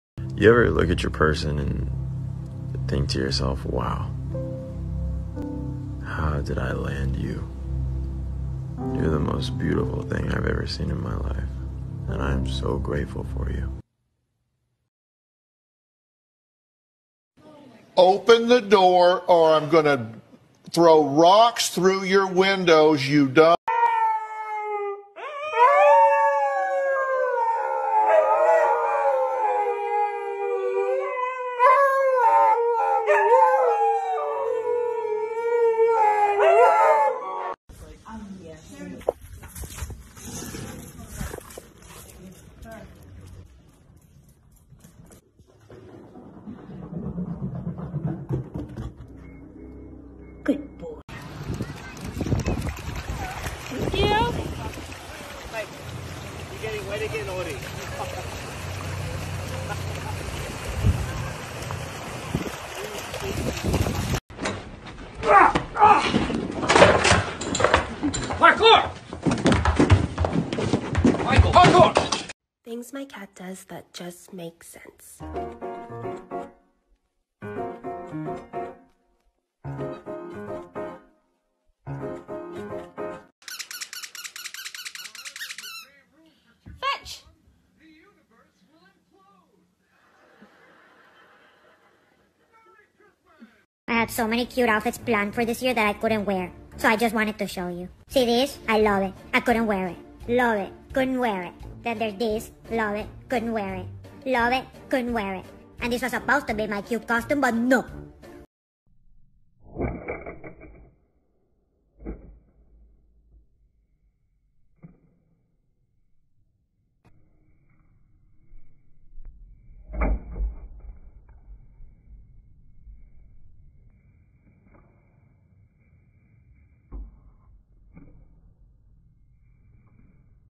Your daily dose of dogs and puppies - why we love dogs
nothing brightens your day like a bunch of cute dogs and puppies
Video credit to:  see video text at specific time